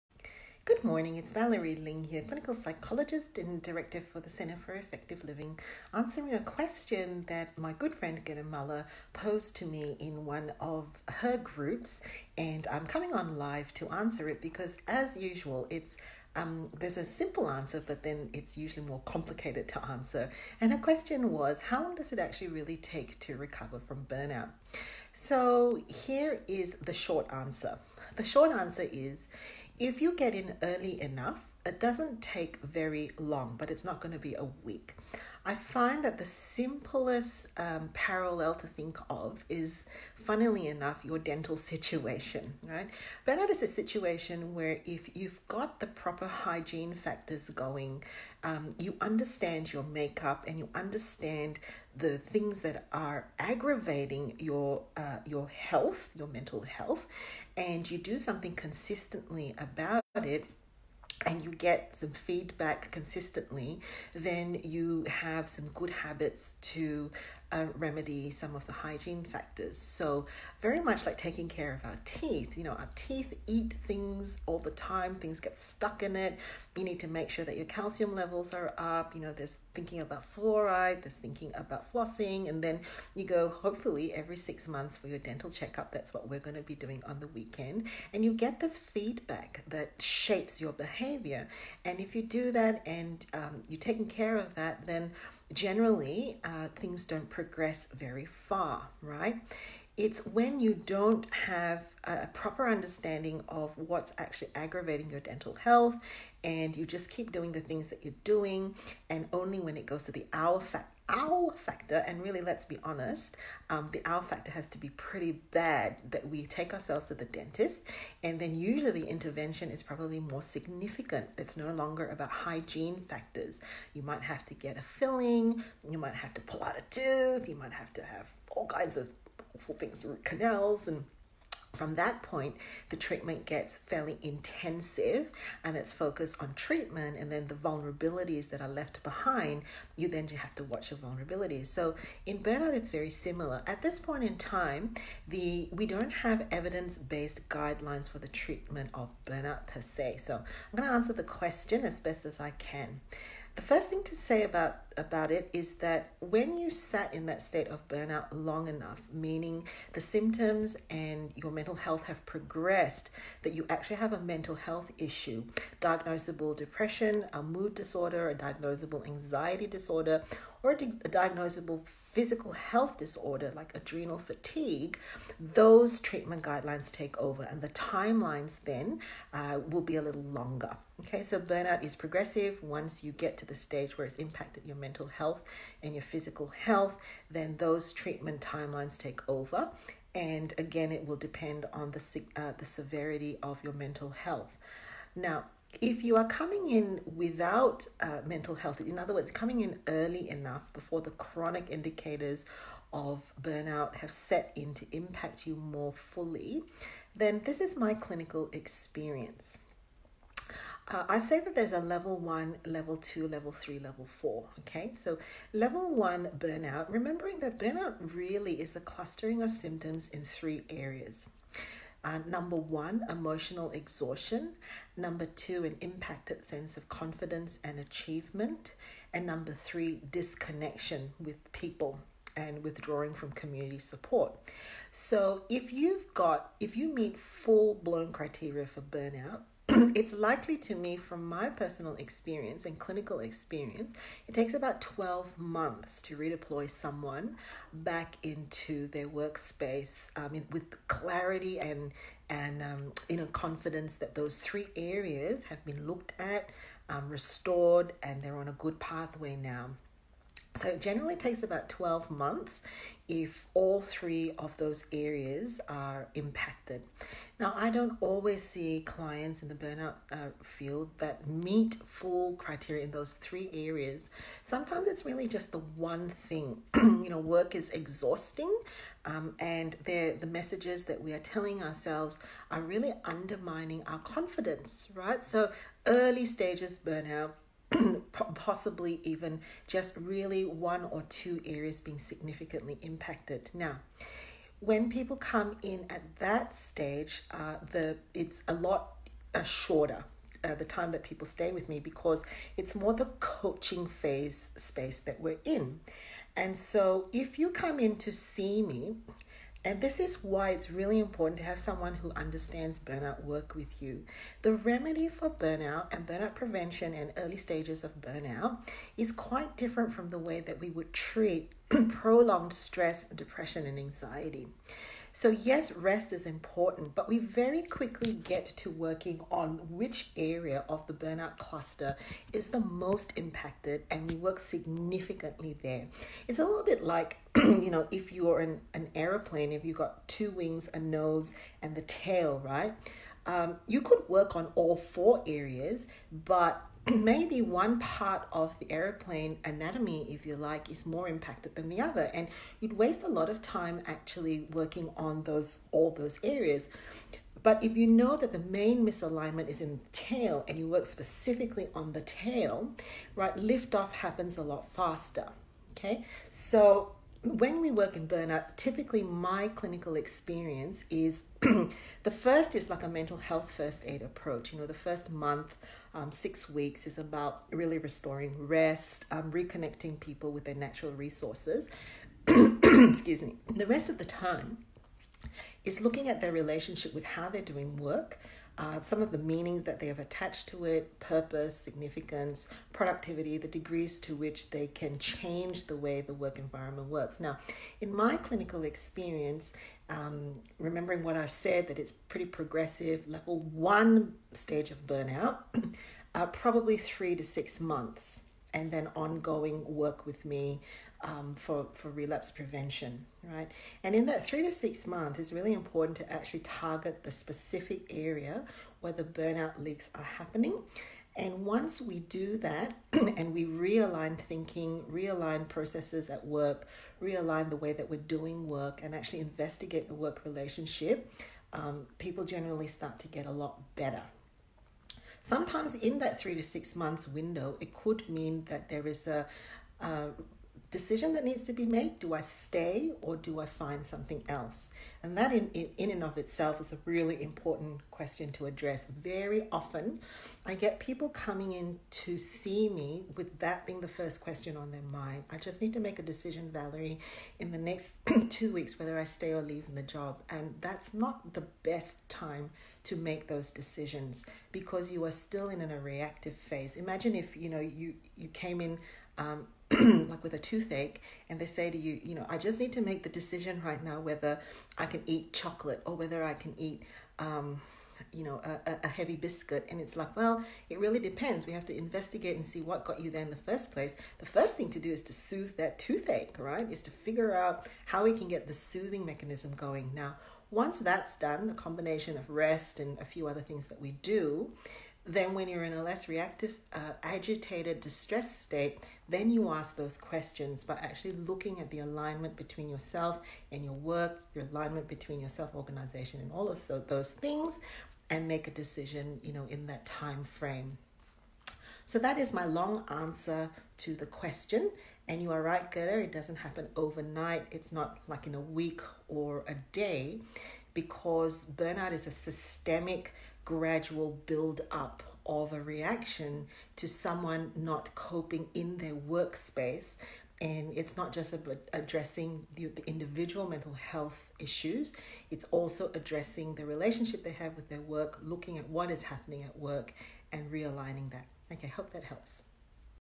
How long does it take to recover from burnout? Answering a question posed by my friend Gerda Muller
How long does it take to recover from burnout?
Answering a question posed by my friend Gerda Muller